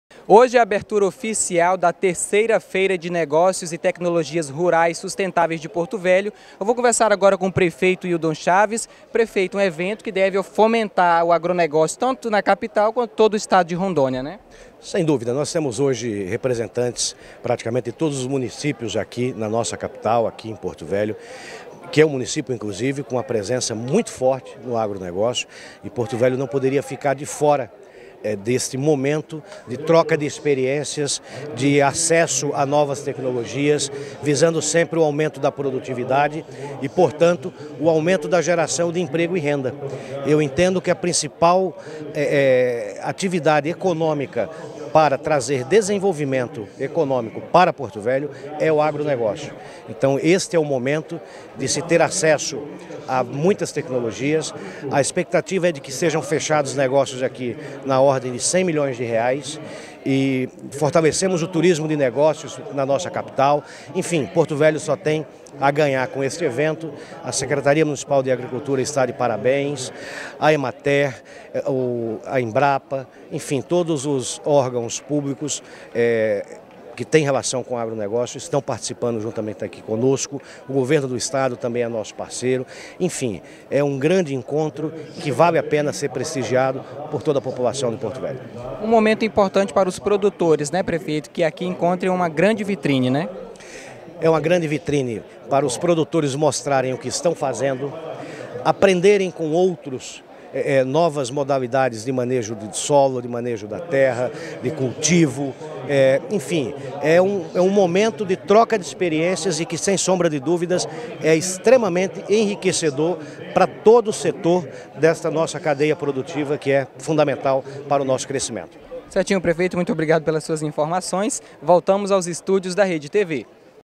Hildon comenta sobre a 3° feira de negócios e tecnologia
Um evento que deve fomentar o agronegócio tanto na capital quanto todo estado de Rondônia.